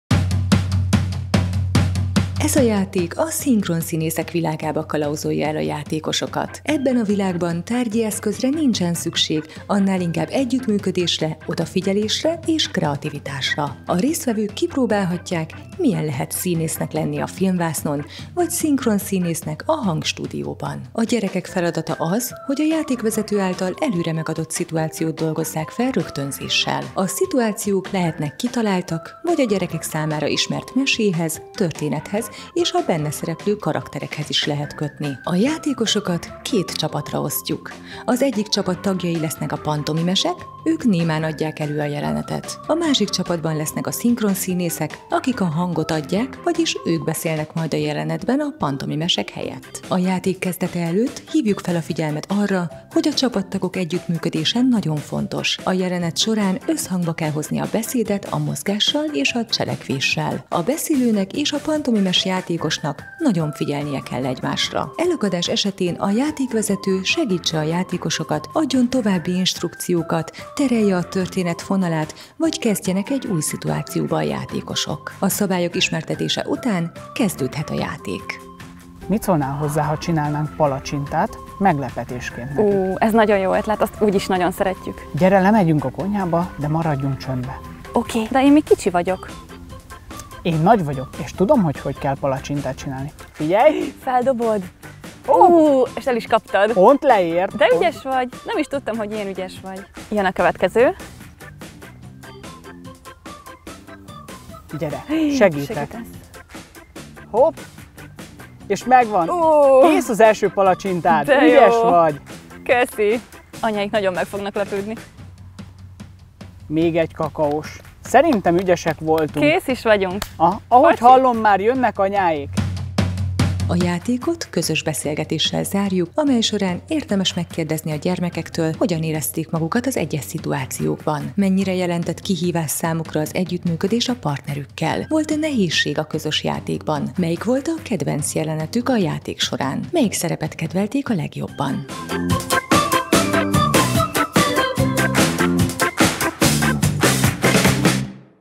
Erzsébet Táborok - JÁTÉK - Szinkronjáték
Ez a játék a szinkronszínészek világába kalauzolja el a játékosokat. Ebben a világban tárgyi eszközre nincsen szükség, annál inkább együttműködésre, odafigyelésre és kreativitásra! A résztvevők kipróbálhatják, milyen lehet színésznek lenni a filmvásznon, vagy szinkronszínésznek a hangstúdióban.